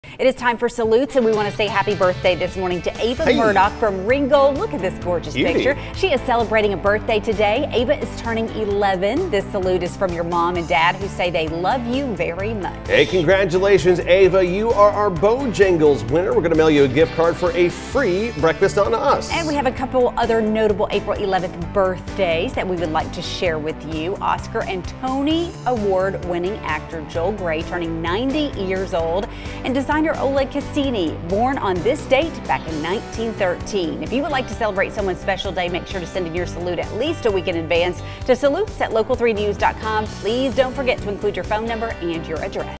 Salutes for Monday, April 11th
We salute everyone who is celebrating an anniversary or a birthday today!